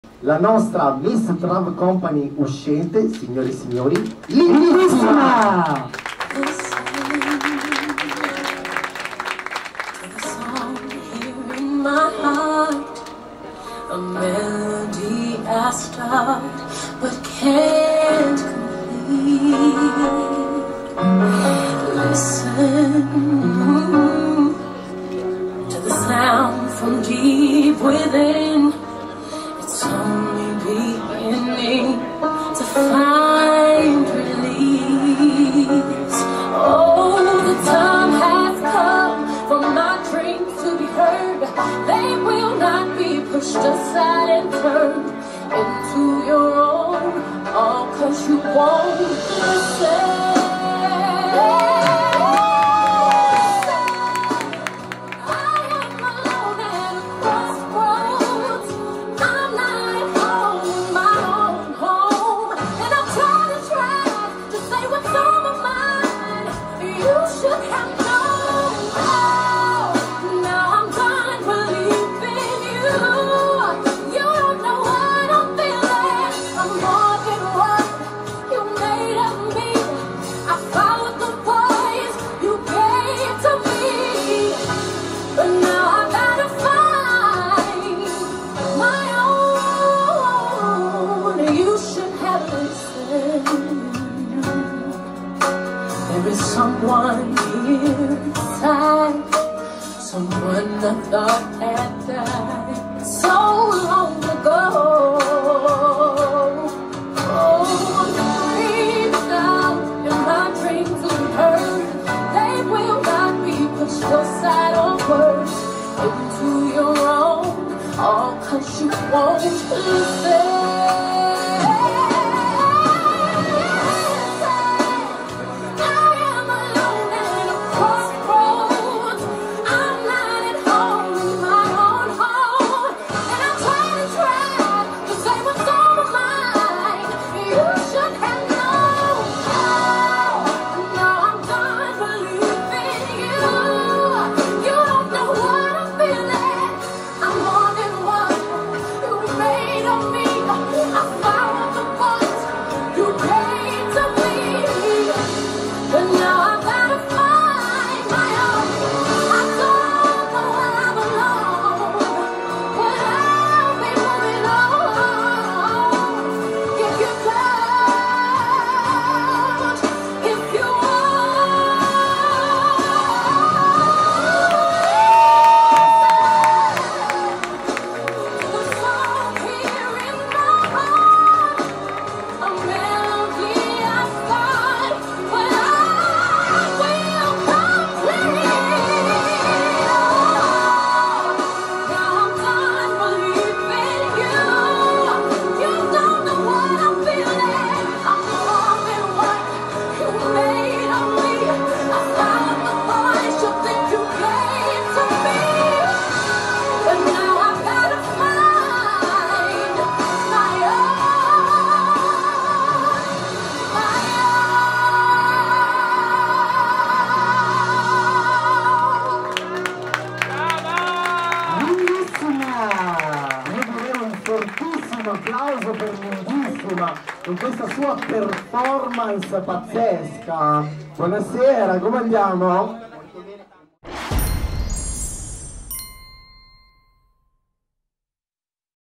Spettacolo di Linda al Miss Trav Company 2022
Spettacolo di Linda del concorso Miss Trav Company 2022,

svolto giorno 11 Giugno 2022, al Nautilus Club Privé di Milano